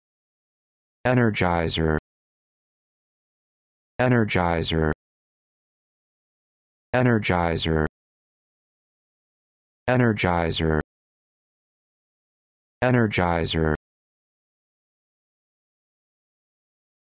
How to Pronounce Energizer
This video shows you how to pronounce Energizer